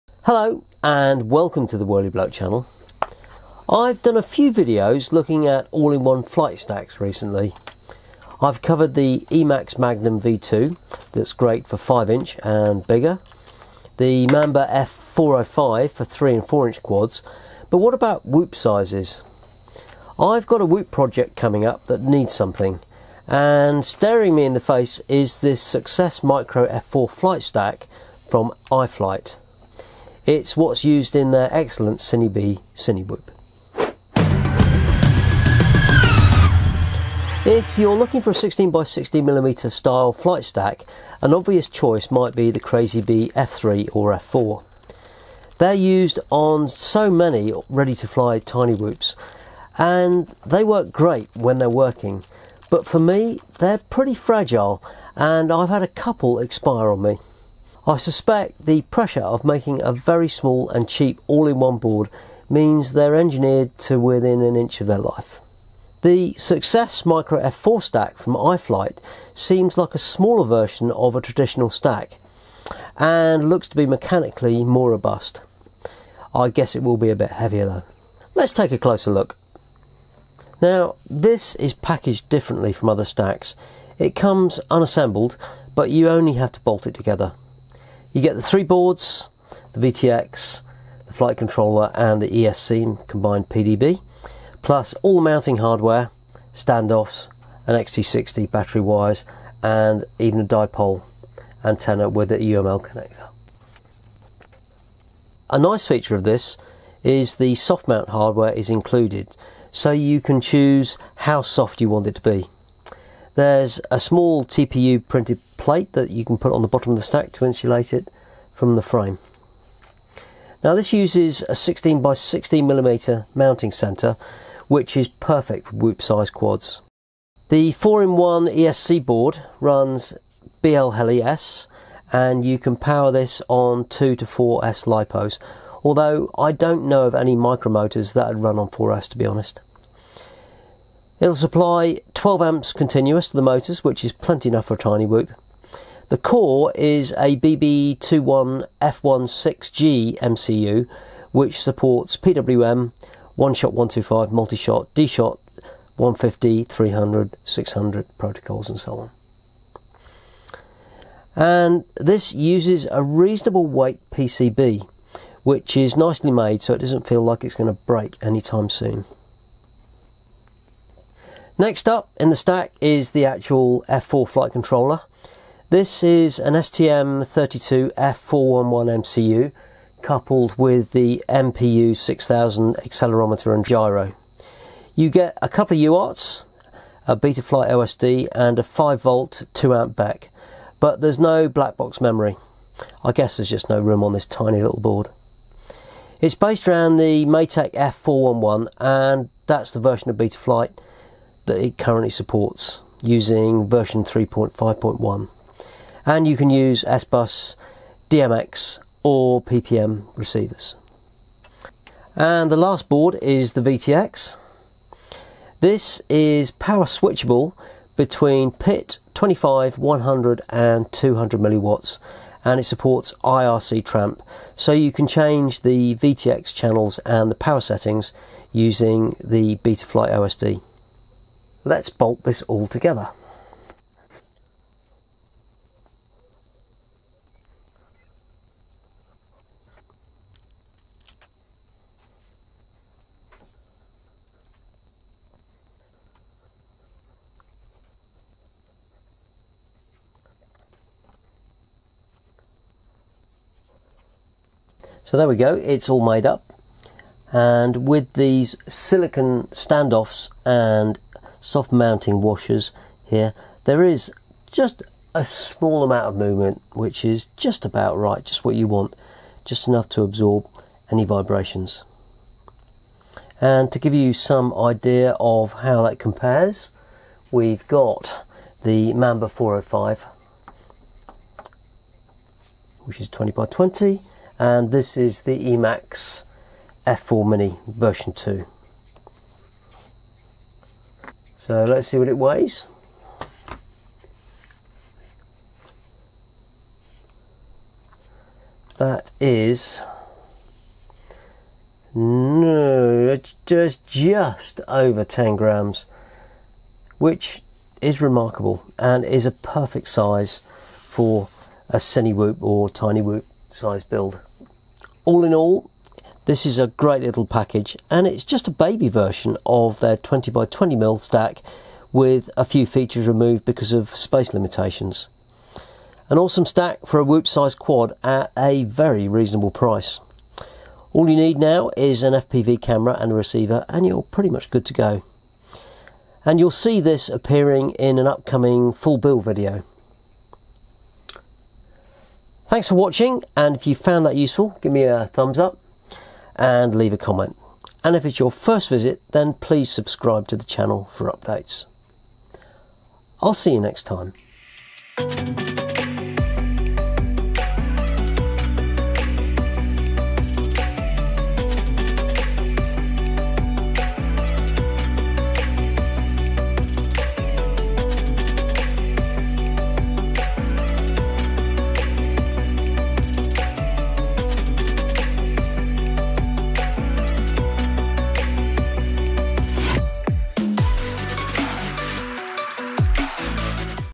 iFlight SucceX Micro 16×16mm F4 V2 Flight Tower Stack 2-4S // 200MW VTX // Full Review
Designed for micro FPV quadcopters like the Cinewhoop and  Toothpick, the mini iFlight Succex stack V2 improves on the popular V1 design.  It uses 16x16mm mounting centers and packs a fully featured Succex F4 flight controller, 4x 12A BLheli_S ESC and a switchable 200mW VTX with IRC Tramp support.

Specifications:
STM32F411
Input voltage 2-4S LIPO
Gyro: MPU6000
2x UART ports
4x motor outputs
OSD inbuilt
No Blackbox FLASH
BEC 5V 2A cont.
Size: 21mm x 21mm
Mounting: 16 x 16mm
Weight: 2.5g
BF target : MATEKF411
SucceX Micro 12A Brushless ESC

Firmware: BlheliS
Continuous current: 12A
MCU: BB21F16G
BEC : None
ESC protocol: pwm, oneshot125, multishot, dshot150, 300, 600, etc.
Size: 23mm*21mm
Mounting  16x16mm
Weight: 2.7g
SucceX Micro VTX

Power : PIT/25/100/200mW
Antenna interface : IPEX (UFL)
Telemetry: IRC Tramp
Size: 17mm*27mm
Mounting: 16x16mm
Weight: 1.6g

Included:
1 x SucceX F4 Flight controller
1 x 12A Brushless ESC
1 x micro VTX